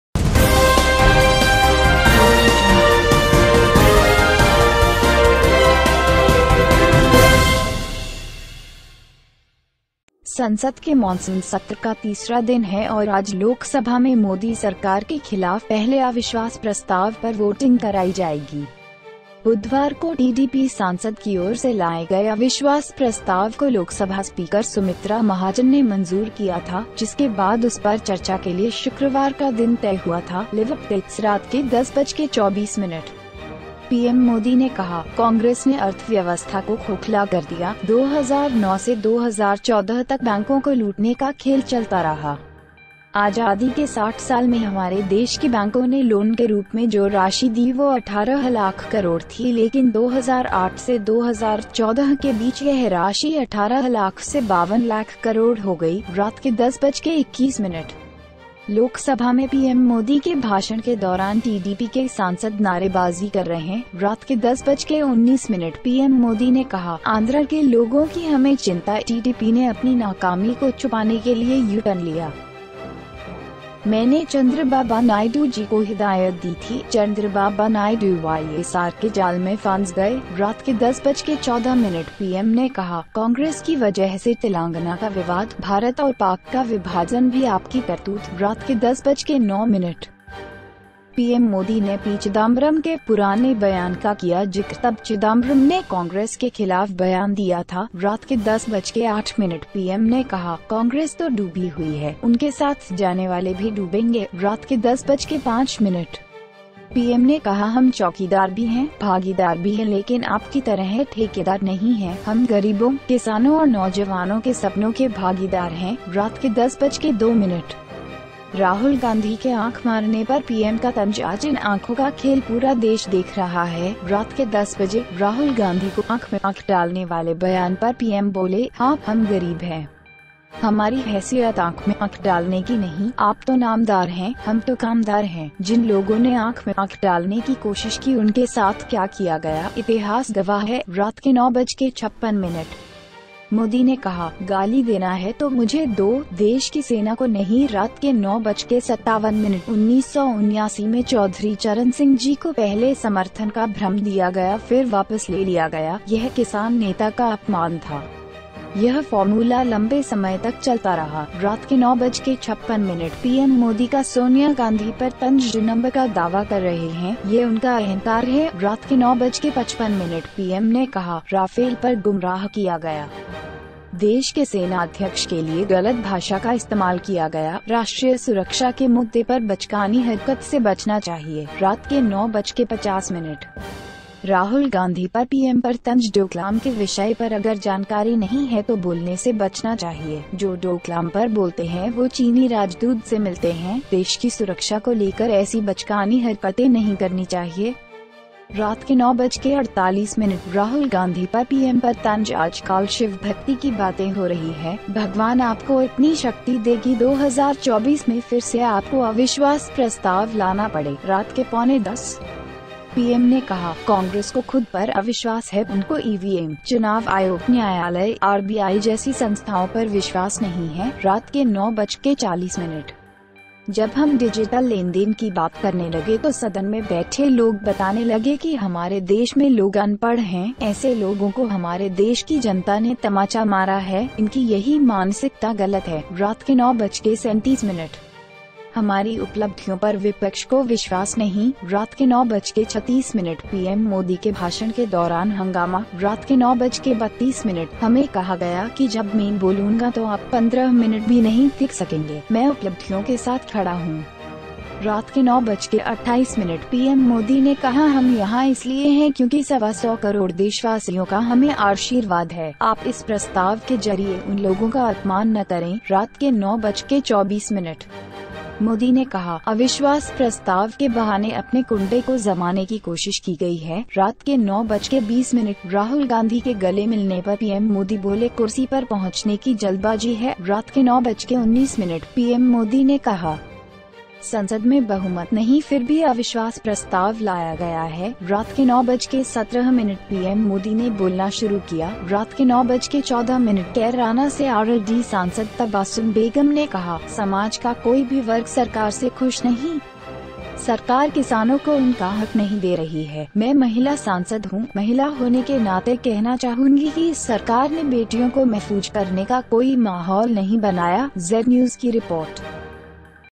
Narendra Modi speech on No Confidence Motion in Parliament.
LIVE: PM Modi's taunts on Rahul - childish movements do not do well, bring seriousness

Parliament is the third day of the monsoon session and today in the Lok Sabha, voting on the first non-confidence motion against the Modi government will be voted. On Wednesday, the motion was taken by Lok Sabha speaker Sumitra Mahajan, which was decided by the TDP MP on Friday, after which it was decided on Friday.

LIVE UPDATES

10:24 pm: PM Modi said - Congress hollowed the economy. From 2009 to 2014, the game of looting of banks continued. In 60 years of our independence, the amount of loan given by the banks in our country was Rs 18 lakh crore, but between 2008 and 2014, the amount went up from 18 lakh to 52 lakh crore.

10:21 PM: TDP MPs are shouting during PM Modi's speech in the Lok Sabha

10:19 pm: PM Modi said - We worry about the people of Andhra. TDP took a U-turn to hide its failure. I had instructed Chandrababu Naidu Ji Chandrababu Naidu got trapped in the trap of YSR

10:14 PM: PM said - Telangana dispute due to Congress. The partition of India and Pakistan is also your work.

10:09 PM: PM Modi did mention of P Chidambaram's old statement. Then Chidambaram gave a statement against the Congress.

10:08 PM: PM said - Congress has drowned, those who go with him will also be drowned.

10:05 PM: PM said, 'We are also janitor, I am a partner but you are not the contractor like you. We are partners of the dreams of the poor, the farmers and the youth.

10:02 PM: PM's stain on Rahul Gandhi's eyeball - Today the game of these eyes is watching the whole country.

10:00 pm: PM said on the statement that Rahul Gandhi was eyeing in the eye - yes, we are poor. Our status can not be eyed in the eye. You are so nominated, we are the workers. Those who tried to put an eye on the eye did what they did with them. History is the witness.

09:56 PM: Modi said - if you want to abuse, let me, not the army of the country

09:57 PM: In 1979, Chaudhary Charan Singh Ji was confused with first support and then withdrawn. It was an insult to the farmer leader. This Formula has been running for a long time.

09:56 PM: PM Modi's Tanaji on Sonia Gandhi - who is claiming the number, it is his ego.

09:55 PM: PM said - Rafael was misled. Wrong language was used for the Chief of the Army. The issue of national security should be avoided by childish acts.

09:50 pm: If Rahul is not aware of the tan-dalal on Rahul Gandhi, then he should refrain from speaking. Those who speak on the occasion, meet the Chinese ambassador. Such childish acts should not be taken against the country's security.

09:48 PM: Rahul Gandhi is tense on PM - things are going on for Shiva devotion today. May God give you so much power that in 2024 you have to bring a no-confidence motion again.

09:45 PM: PM said - Congress has disbelief on itself. They do not believe in organizations like EVM, EC, court and RBI.

09:40 PM: When we started talking about digital transactions, people sitting in the house began to tell that people in our country are illiterate. Such people have killed the people of our country. Their mentality is wrong.

09:37 pm: The opposition does not believe in our achievements.

09:36 PM: Commotion during PM Modi's speech

09:32 PM: We were told that when I speak, you will not be able to stay for 15 minutes. I am standing with achievements.

09:28 PM: PM Modi said: 'We are here because we are blessed with the blessings of 100 million people.' You do not insult those people through this proposal.

09:24 PM: Modi said - An attempt has been made to rehabilitate his family.

09:20 pm: PM Modi said, when Rahul Gandhi got his hug - he is in a hurry to reach the chair.

09:19 PM: PM Modi said, no motion of no confidence in the parliament has been brought yet.

09:17 pm: PM Modi started speaking

09:14 PM: RLD MP from Karana, Tabasum Begum said that no section of society is happy with the government. The government is not giving the farmers the right to them. I am a woman MP. As a woman, I would like to say that this government has not made any ambition to entertain daughters.